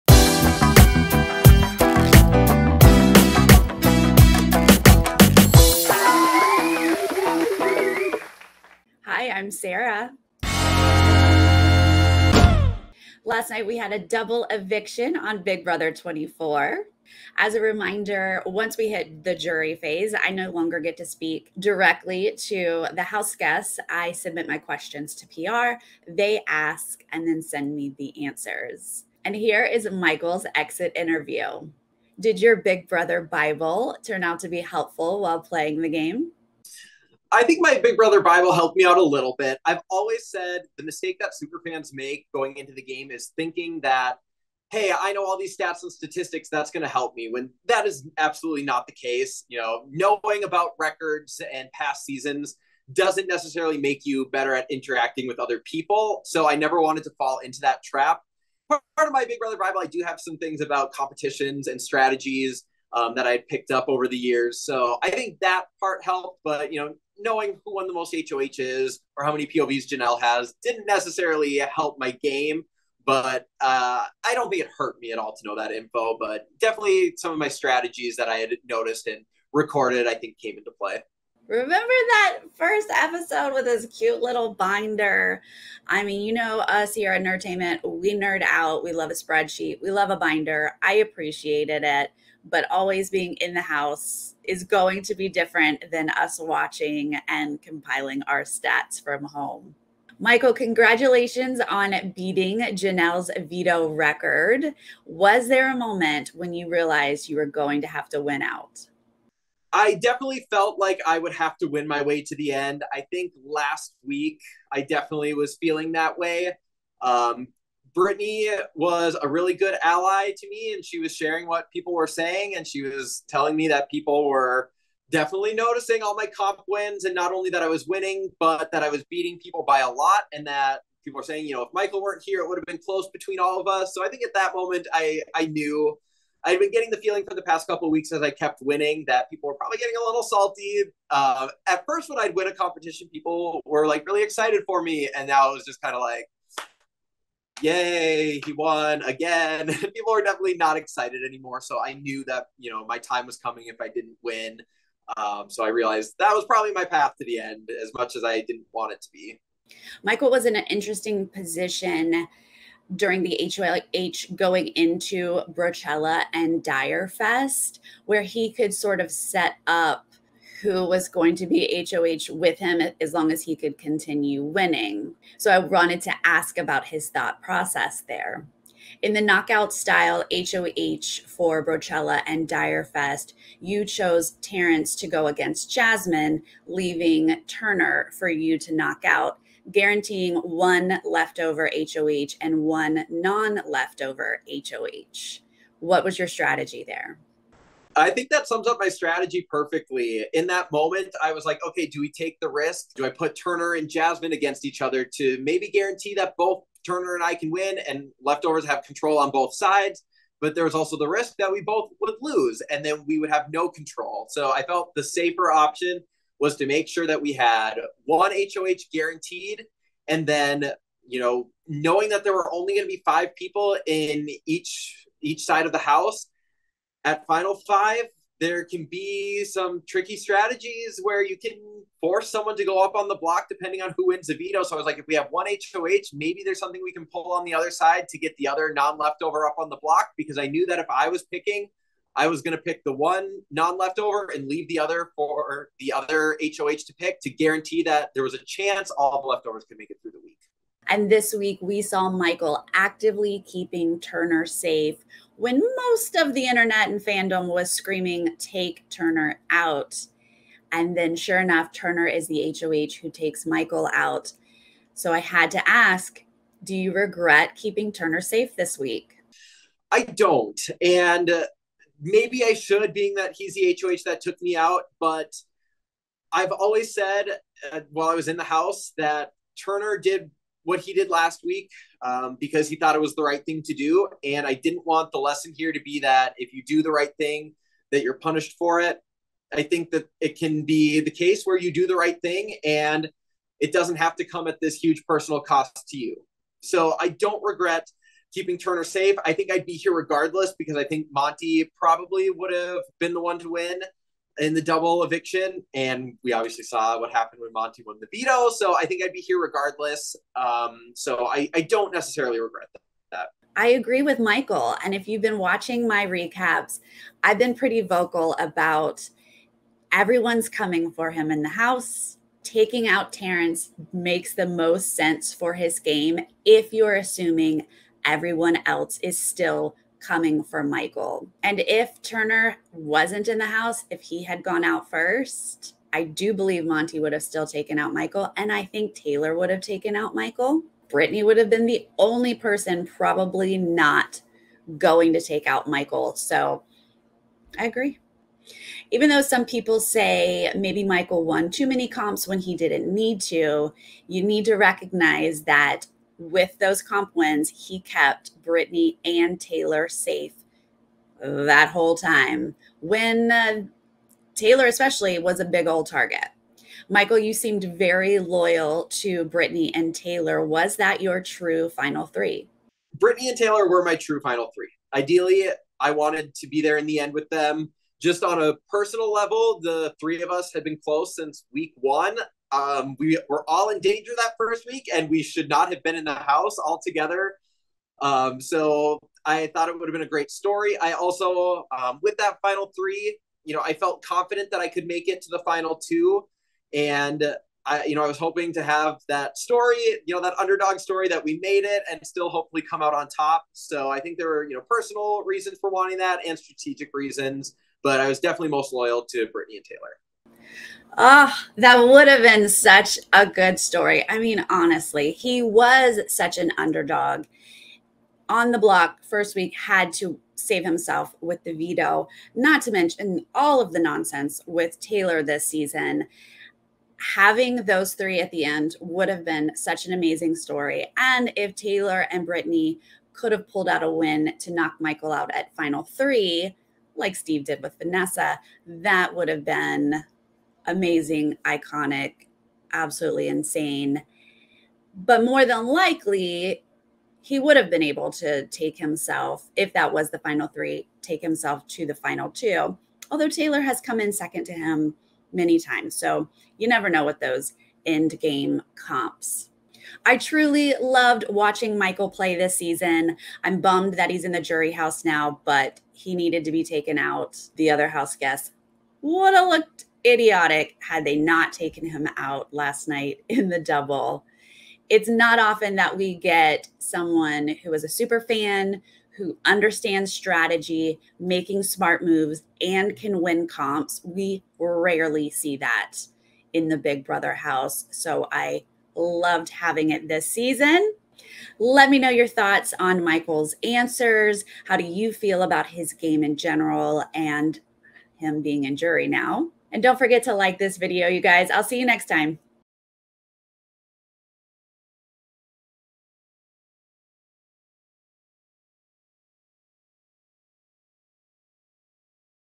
BIG BROTHER 24 EXIT INTERVIEW / MICHAEL BRUNER, WEEK 9 DOUBLE EVICTION⎰Nerdtainment
Sarah is talking to the Houseguest evicted in the Double Eviction in Week 9 of Big Brother 24. She is talking to Michael about his strategy, his loyalties and regrets.

PERSONAL TWITTER: @SarahAtwood
PERSONAL IG: @TheSarahAtwood